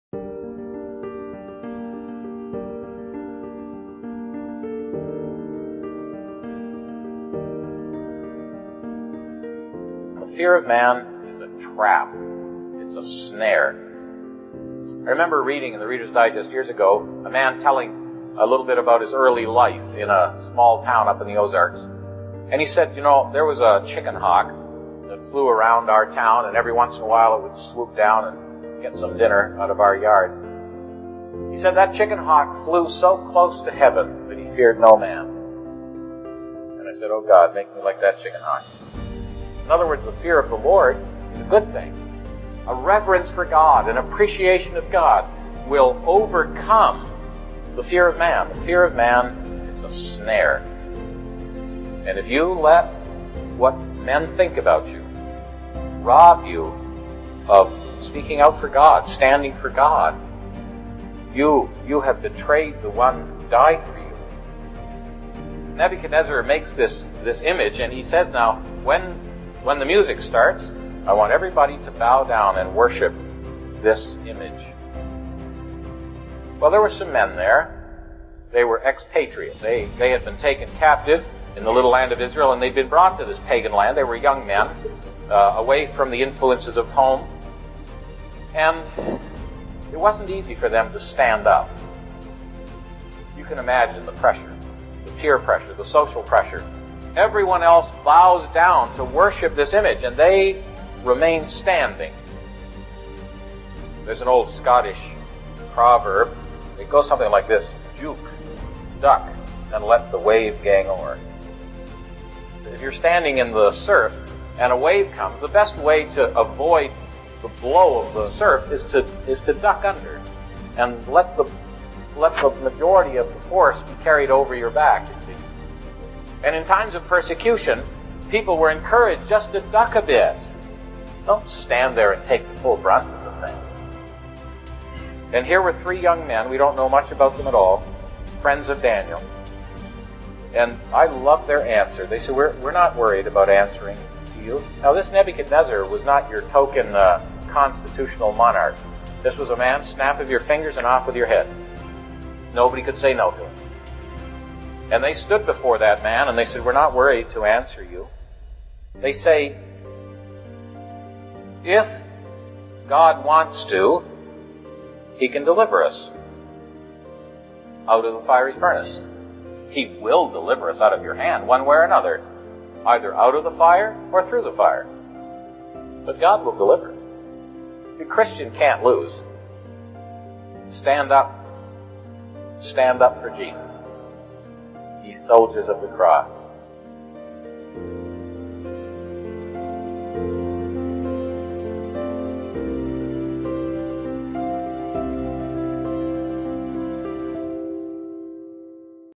Mini-Message Monday: Flying High!
Mini-Message Monday: Flying High! (3 mins) — Forget about chickens crossing  roads. Here's a great lesson about a chicken hawk soaring the skies and how you can reach new spiritual heights in 2024!